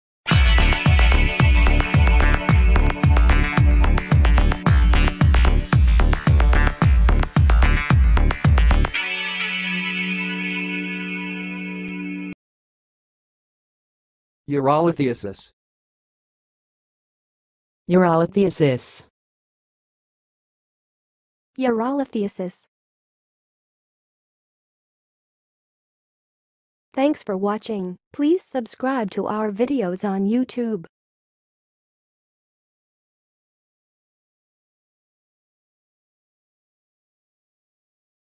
UROLITHIASIS - HOW TO PRONOUNCE IT?
UROLITHIASIS - full guide about Urolithiasis in 2023

How to pronounce 'calcium oxalate' correctly?
What is the correct pronunciation of 'ureter'?
Can you provide alternative ways to pronounce 'renal function'?
UROLITHIASIS Meaning And Definition:
Urolithiasis, also known as kidney stones, refers to the formation of solid mineral and acid salt deposits in the urinary tract.
These stones can be found in various parts of the urinary system, including the kidneys, ureters, bladder, and urethra.
Urolithiasis typically occurs when certain substances in the urine, such as calcium, oxalate, and uric acid, become highly concentrated and crystallize.
The resulting stones can range in size from tiny grains to large stones that can obstruct the urinary tract and cause severe pain and discomfort.
UROLITHIASIS Pronunciation:
To pronounce 'urolithiasis', break it down as 'yoo-roh-lih-THY-uh-sis'.
Another variant pronunciation is 'yor-oh-lih-THY-uh-sis'.
Use UROLITHIASIS In Your Speech:
1. He suffered from severe pain due to urolithiasis
2. The doctor recommended a surgical procedure to remove the kidney stones caused by urolithiasis
3. She experienced frequent urination and blood in her urine, which are common symptoms of urolithiasis
Please answer in comments for the most popular UROLITHIASIS questions:
1. What are the risk factors for developing urolithiasis?
2. How are kidney stones formed in urolithiasis?
3. What are the non-surgical treatment options for urolithiasis?
4. Can urolithiasis​ lead to kidney damage?
5. What dietary changes can help prevent urolithiasis?